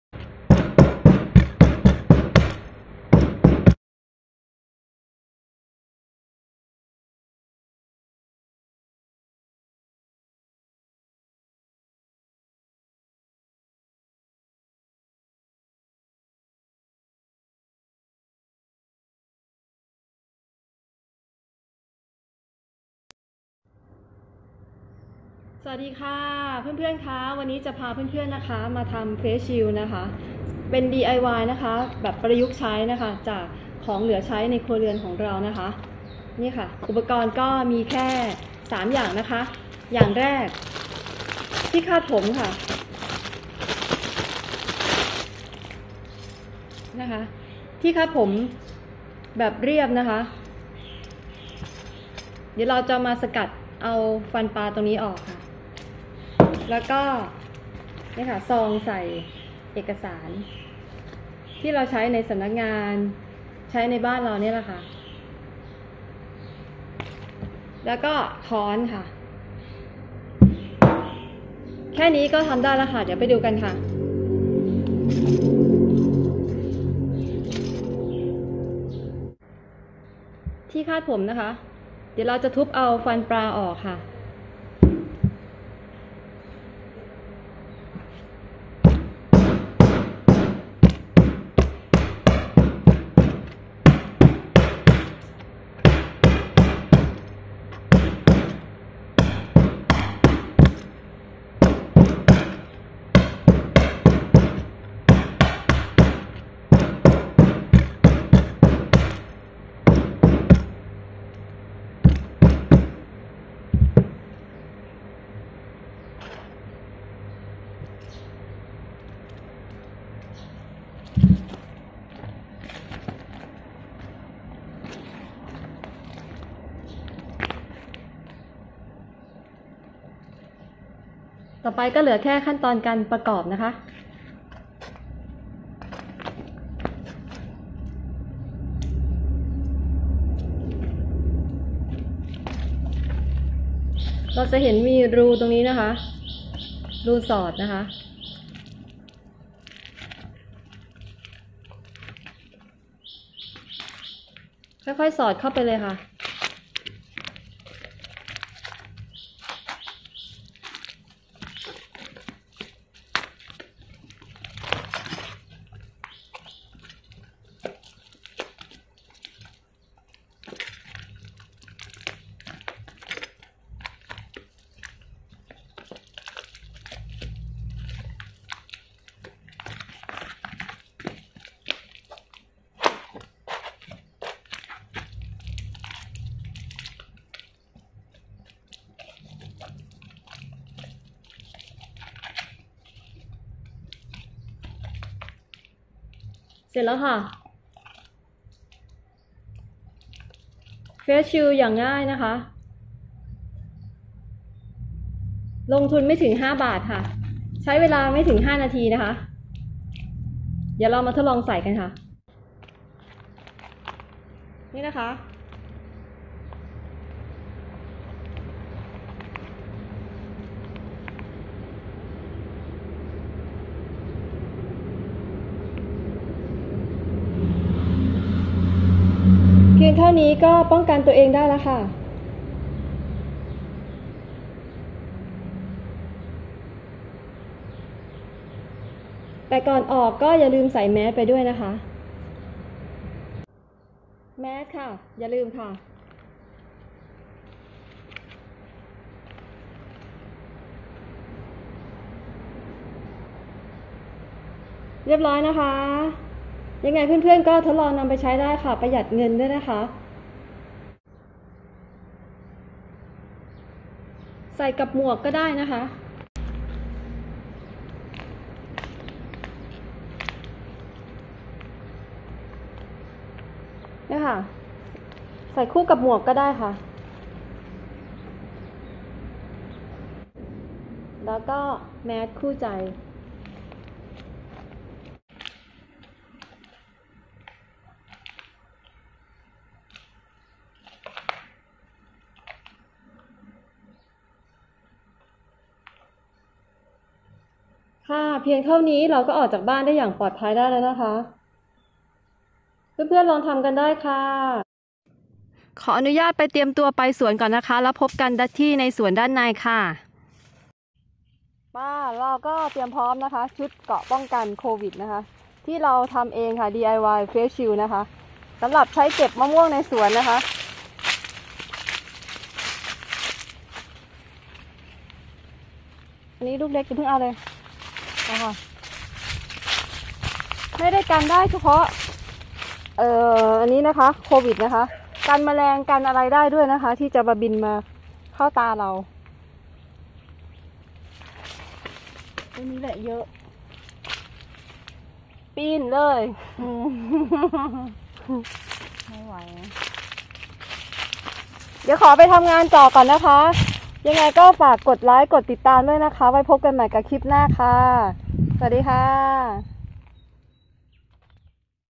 Face Shield DIYหน้ากากกันละอองฝุ่นน้ำ/or-chan
Face Shield DIY หน้ากากกันฝุ่นละอองน้ำทำง่ายด้วยงบ 3บาท